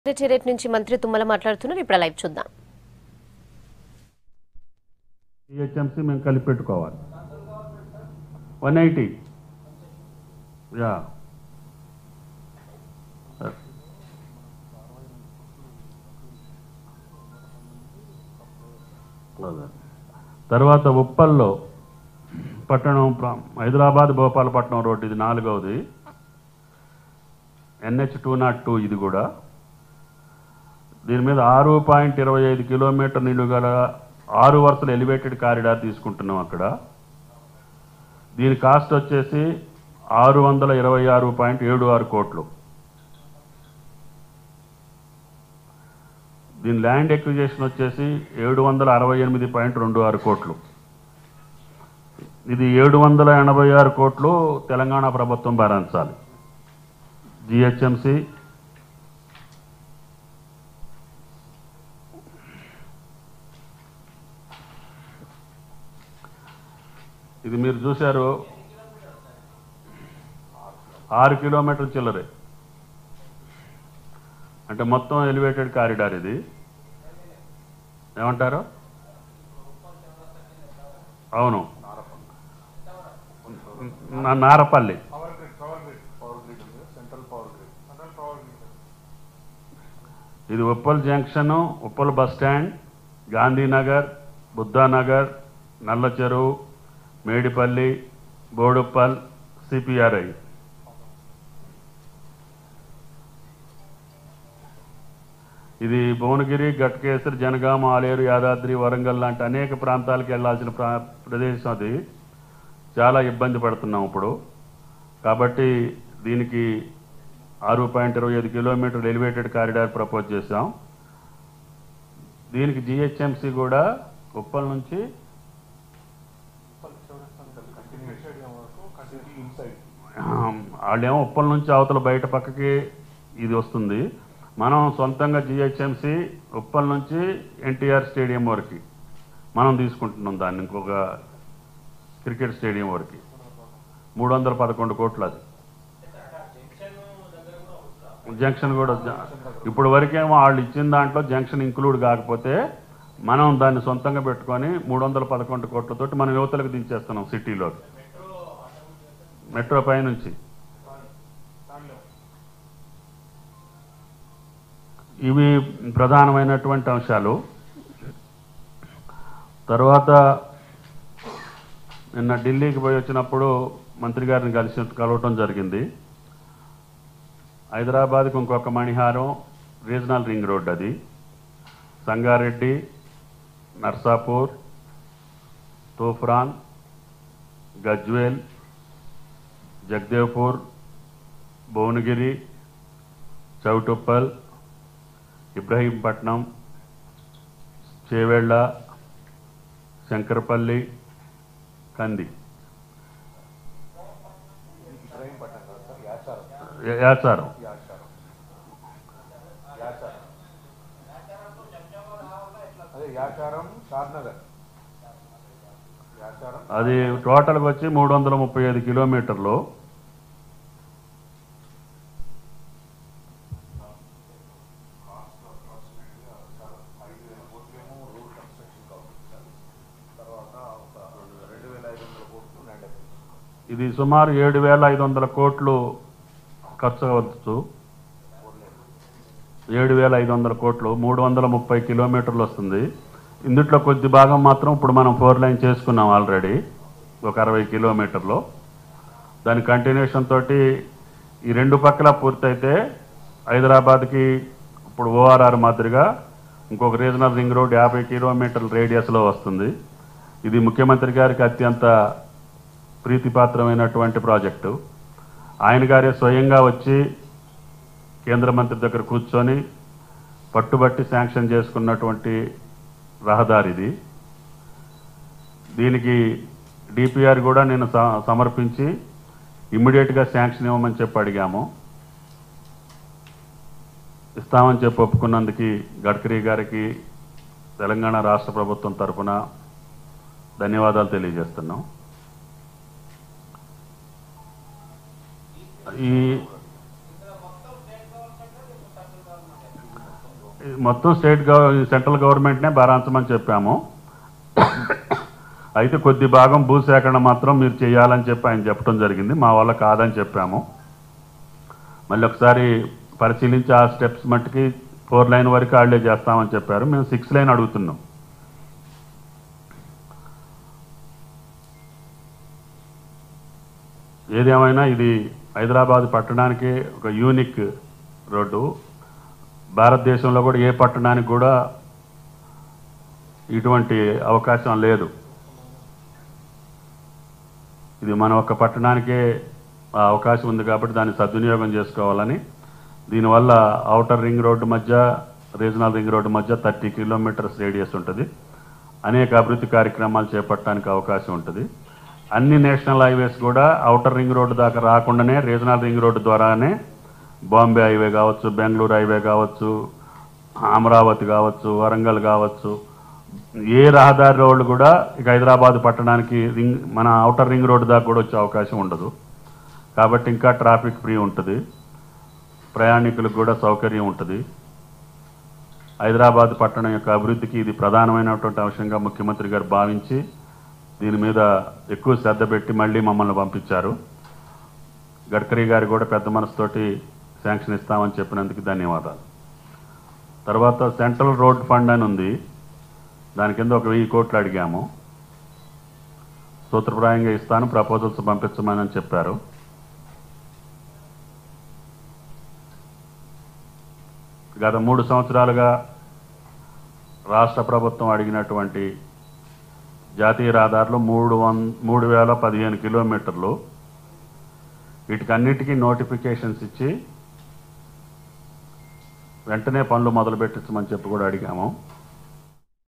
Minister Thummala Speaks On Roads Development In Telangana | TRS Ministers Meets Nitin Gadkari | V6
Minister Thummala Speaks On Roads Development In Telangana | TRS Ministers Meets Nitin Gadkari.